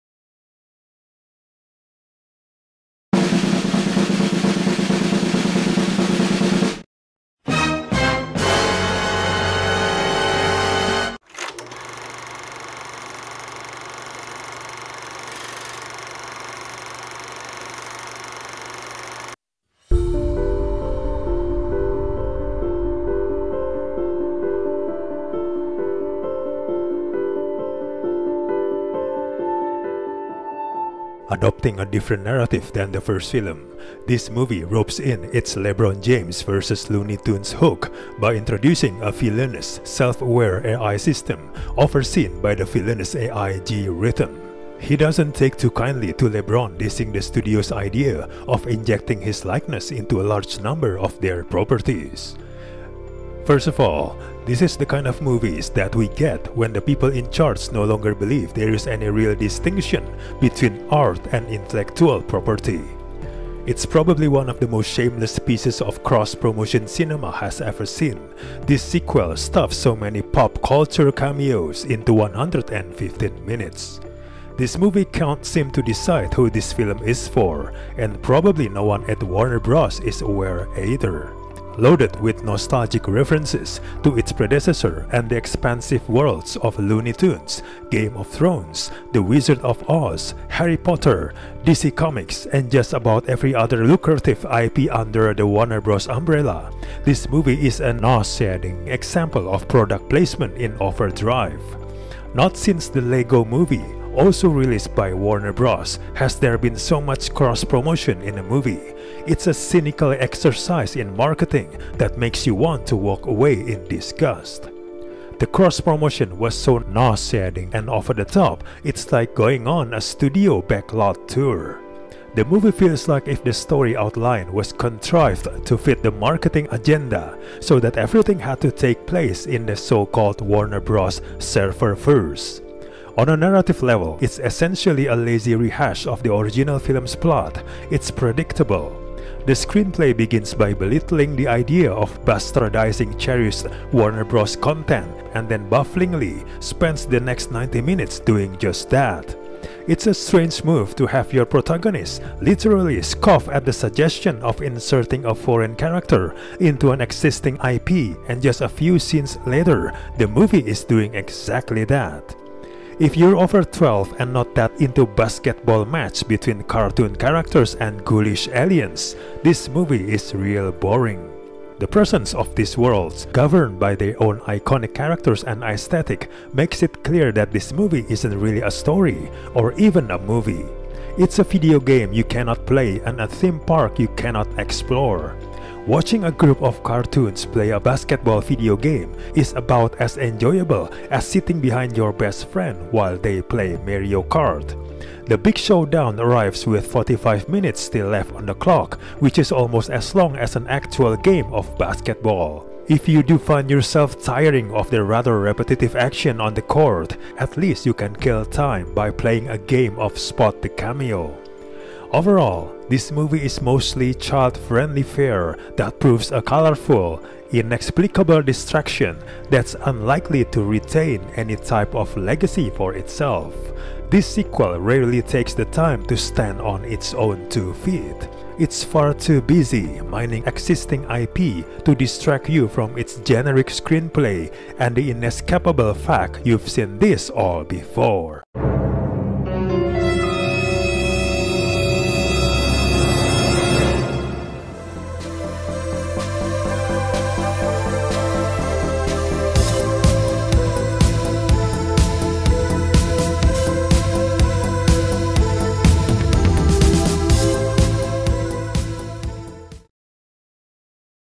Space Jam: A New Legacy LeBron James Review - Bad Movie Reviews
Space Jam 2: A New Legacy Review.

A rogue artificial intelligence kidnaps the son of famed basketball player LeBron James, who then has to work with Bugs Bunny to win a basketball game.

Directed by : Malcolm D. Lee.

Starring :

- LeBron James
- Don Cheadle
- Cedric Joe
- Khris Davis
- Sonequa Martin-Green
- Ceyair J Wright
- Harper Leigh Alexander
- Xosha Roquemore
- Stephen Kankole
- Jalyn Hall
- Wood Harris
- Jordan Thomas